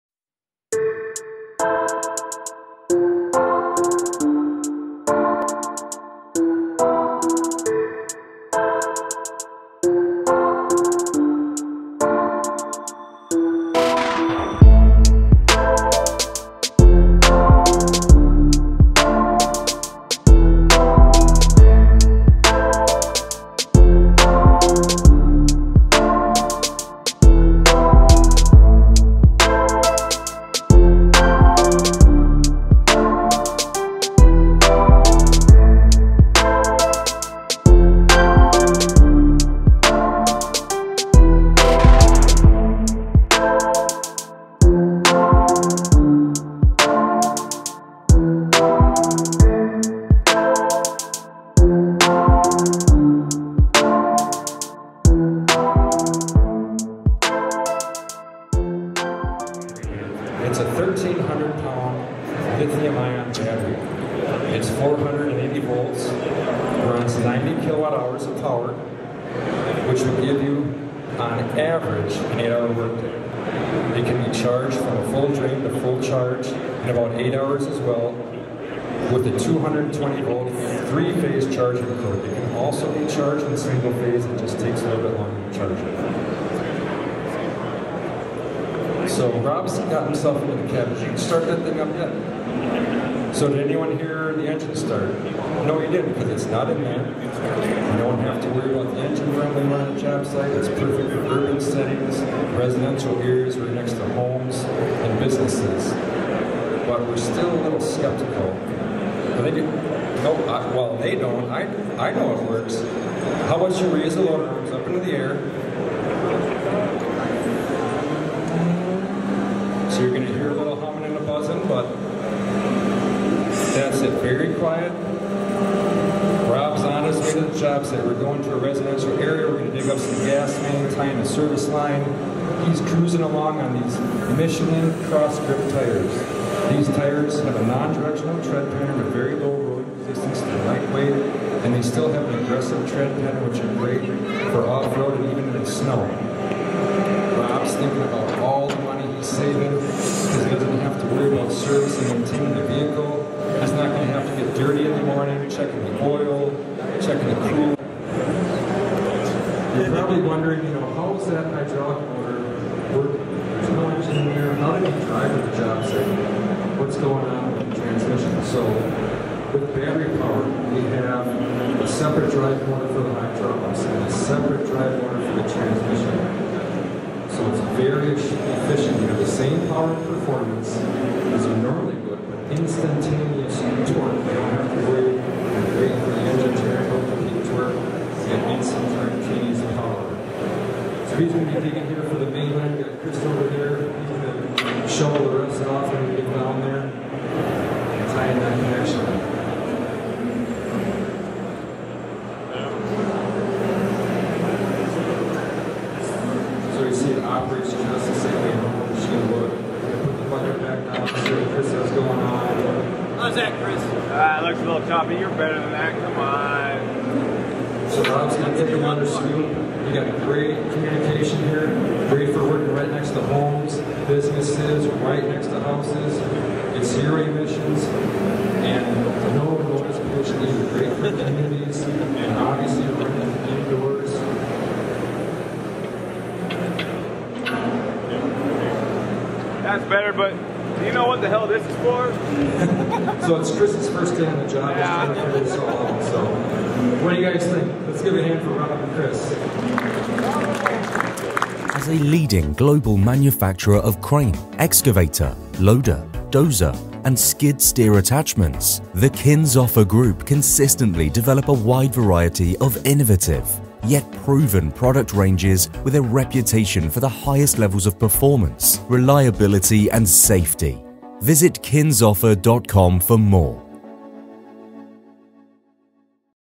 CASE Launch Project ZEUS at ConExpo 2020! (first ever electric battery powered backhoe)
We joined Case in the North Hall of ConExpo to get the first look at their latest innovation; Project Zeus. Following in the foot steps of their last electric concept, Project Tetra, this is the first ever electric battery powered back hoe. The 580E is powered by a 1,300lb lithium ion battery at 480 volts and capable of 90kW/hour for an 8 hour work day.

Our channel is dedicated to finding out the best facts figures and action about the world of heavy machinery.